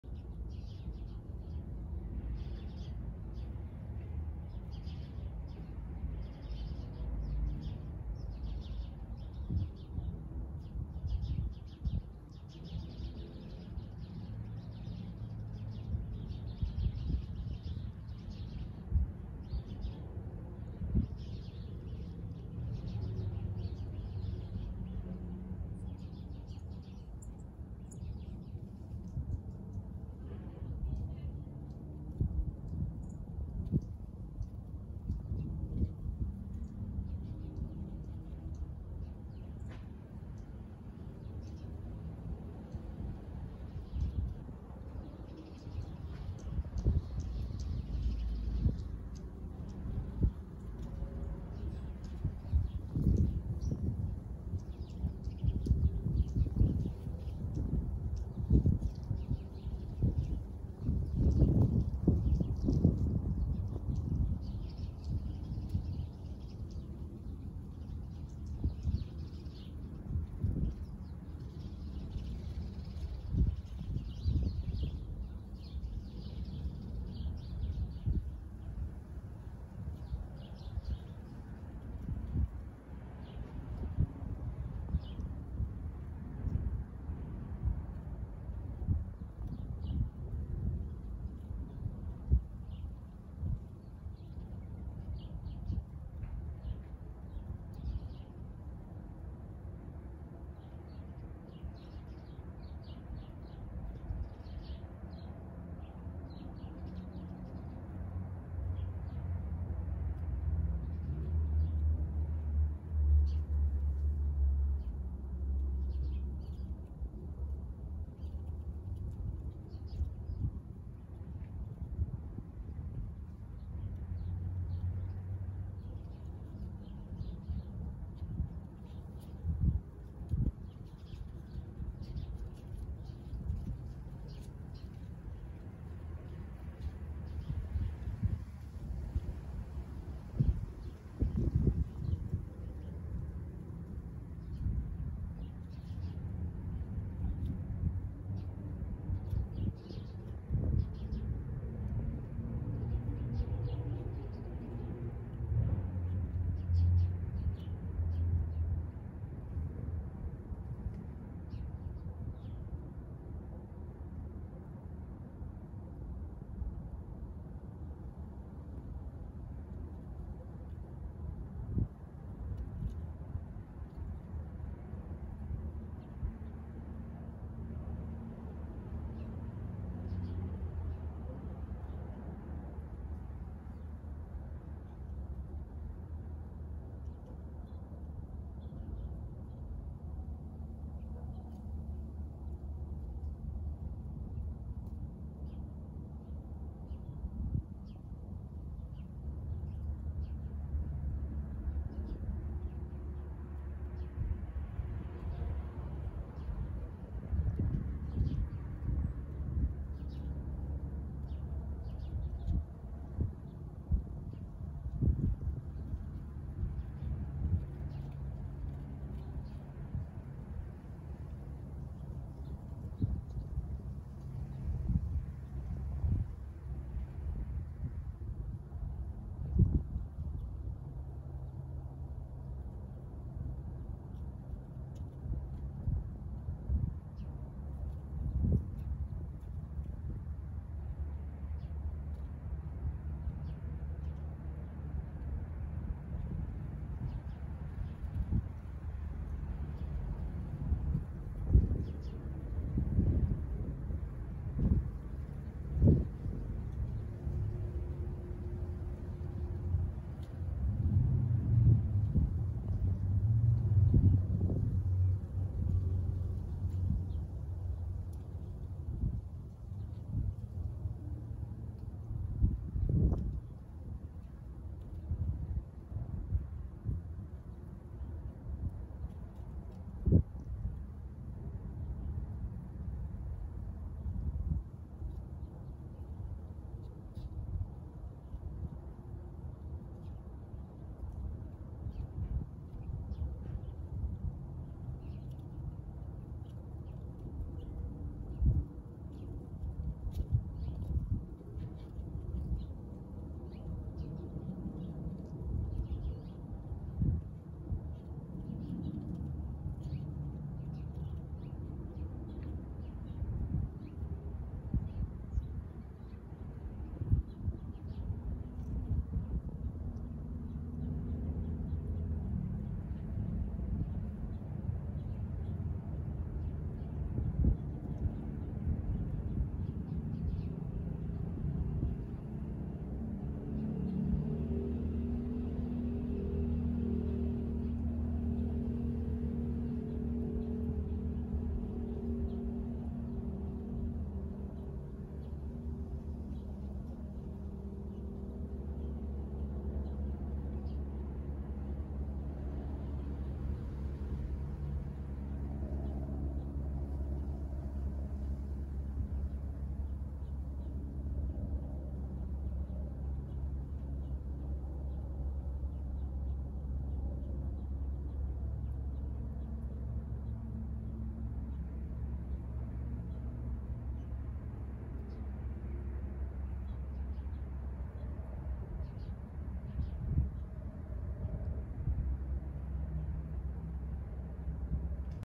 no more leaves
trees in my back yard taking a break from leaf growing to focus on inner strength.